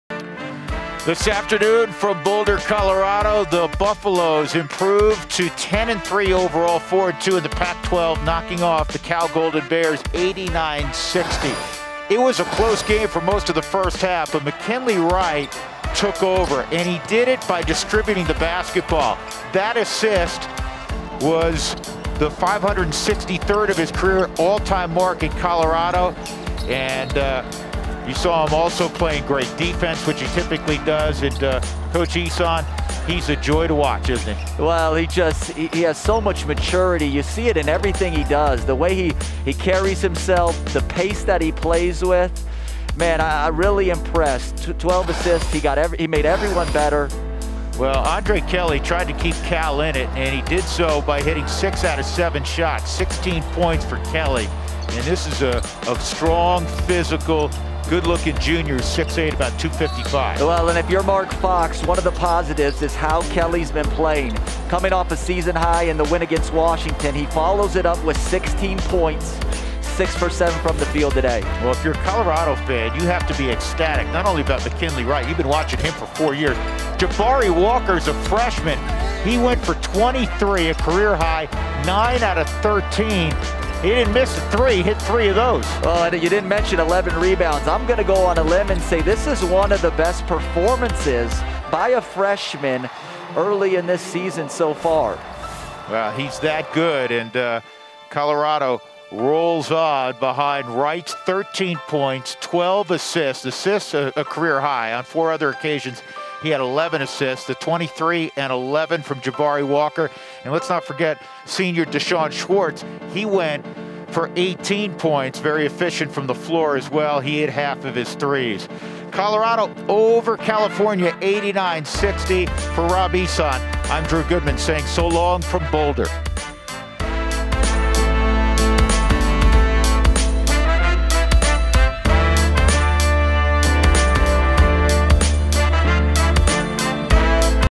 Wright IV becomes CU’s all-time assists leader, notches career-high 12 dimes in win over Cal
Pac-12 Networks' Drew Goodman and Robert Ehsan recap Colorado men's basketball 89-60 victory over California to improve to a perfect 4-0 in Boulder this season. Senior guard McKinley Wright IV moved into first all-time on Colorado's all-time assists leaderboard, passing Jay Humphries' previous record-leading 563 assists. Wright IV also notched a career-high 12 assists and 13 points for the 13th double-double of his career. Freshman forward Jabari Walker scored a career-high 23 points and added 11 rebounds for his second straight double-double for the Buffs. Colorado moves to 10-3 overall and 4-2 in the conference, while Cal falls to 6-8 overall and 1-6 in Pac-12 play.